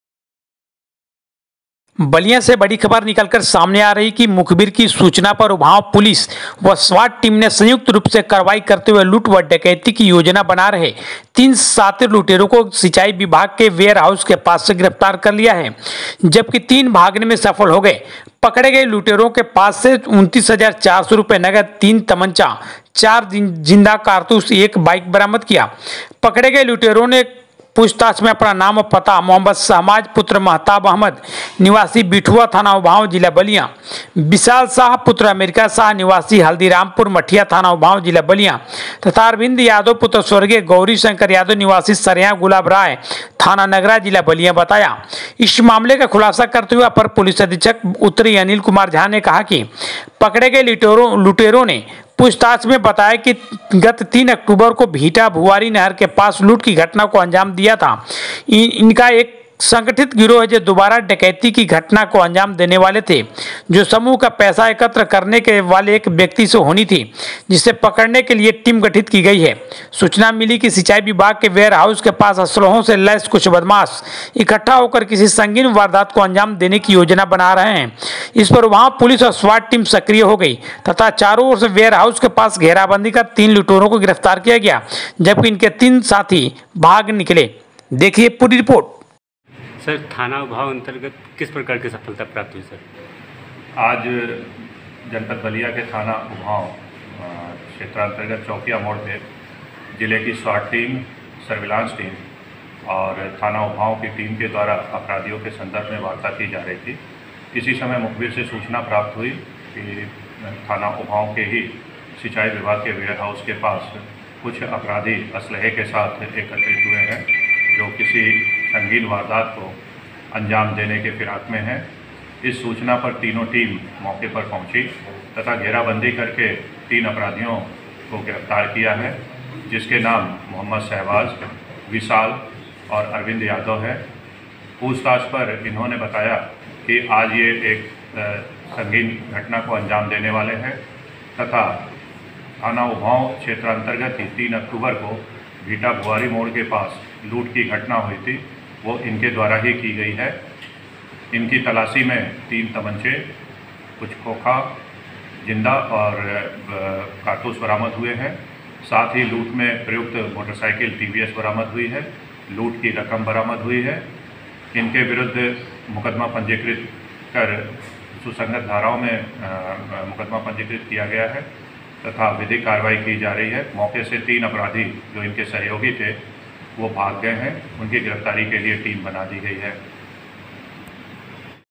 बलिया-लूट,डकैती की योजना को अंजाम देने वाले तीन शातिर लुटेरे गिरफ्तार,तीन हुये फरार।
बलिया में लूट,डकैती की योजना को अंजाम देने वाले तीन शातिर लुटेरे गिरफ्तार,तीन हुये फरार

खबर बलिया से है मुखबीर की सूचना पर उभांव पुलिस व स्वाट टीम ने संयुक्त रूप से कार्रवाई करते हुए लूट व डकैती की योजना बना रहे तीन शातिर लुटेरों को सिंचाई विभाग के वेयर हाउस के पास गिरफ्तार कर लिया। जबकि तीन भागने में सफल हो गए। पकड़े गए लुटेरों के पास से 29400 रुपए नकद, तीन तमंचा, चार जिंदा कारतूस, एक बाइक बरामद किया। पकड़े गए लुटेरों ने पूछताछ में अपना नाम व पता मो शहबाज पुत्र महताब अहमद निवासी बिठुआ थाना उभांव जिला बलिया, विशाल शाह पुत्र अमेरिका शाह निवासी हल्दी रामपुर मठिया थाना उभांव जिला बलिया तथा अरविन्द यादव पुत्र स्व गौरी शंकर यादव निवासी सरयां गुलाब राय थाना नगरा जिला बलिया बताया।

मामले का खुलासा करते हुए अपर पुलिस अधीक्षक उत्तरी अनिल कुमार झा ने कहाकि कि पकड़े गए लुटेरों ने पूछताछ में बताया कि गत तीन अक्टूबर को भींटा भुआरी नहर के पास लूट की घटना को अंजाम दिया था। इनका एक संगठित गिरोह है जो दोबारा डकैती की घटना को अंजाम देने वाले थे जो समूह का पैसा एकत्र करने वाले एक व्यक्ति से होनी थी। जिसे पकड़ने के लिए टीम गठित की गई थी। सूचना मिली कि सिंचाई विभाग के वेयर हाउस के पास असलहों से लैस कुछ बदमाश इकट्ठा होकर किसी संगीन वारदात को अंजाम देने की योजना बना रहे हैं। इस पर उभांव पुलिस व स्वाट टीम सक्रिय हो गई तथा चारों ओर से वेयरहाउस के पास घेराबंदी कर तीन लुटेरों को पकड़ लिया। जबकि इसके तीन साथी भाग निकले।